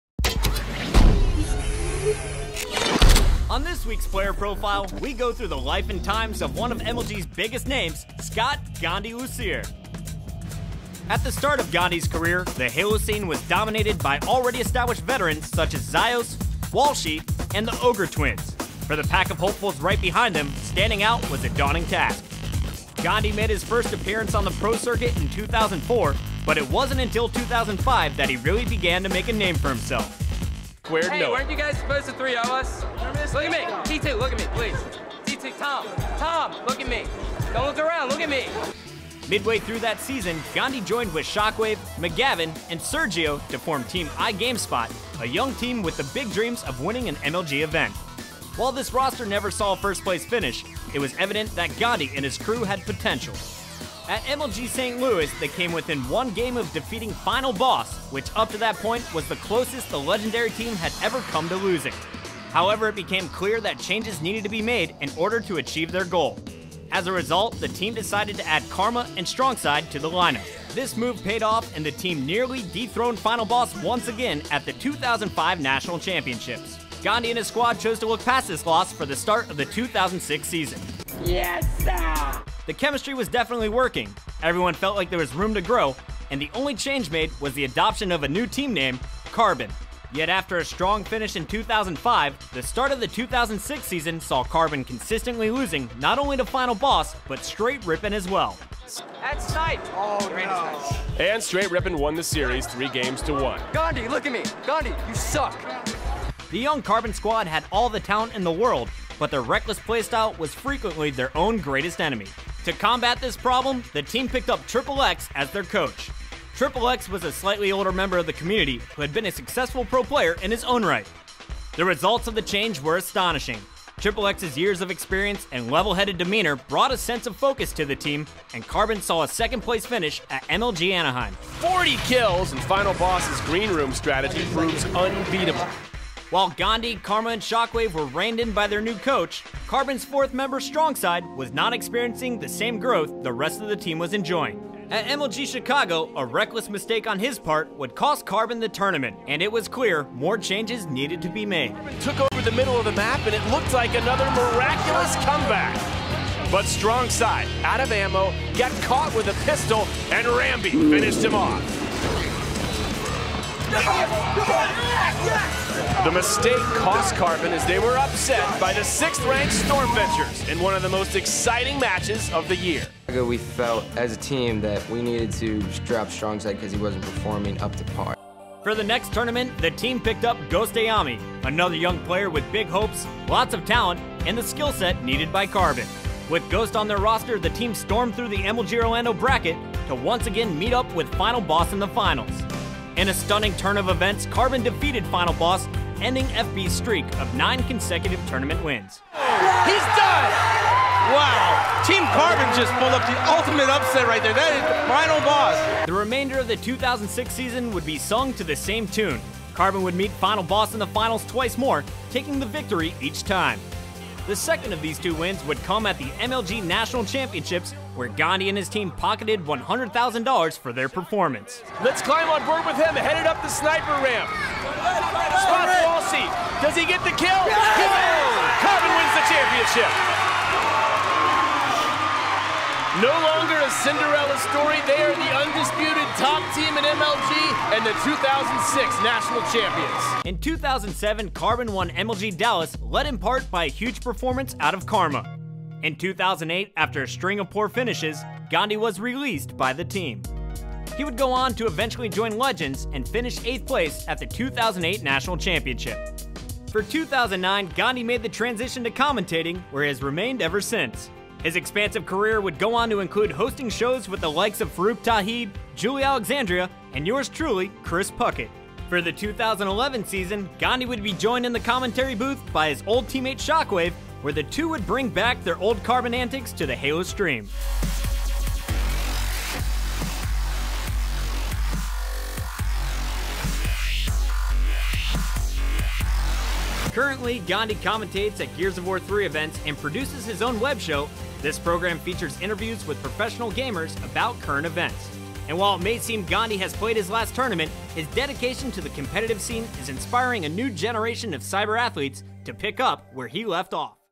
Player Profile: Gandhi (Halo)
In this week's profile we look back on the career of Halo Pro/Commentator Gandhi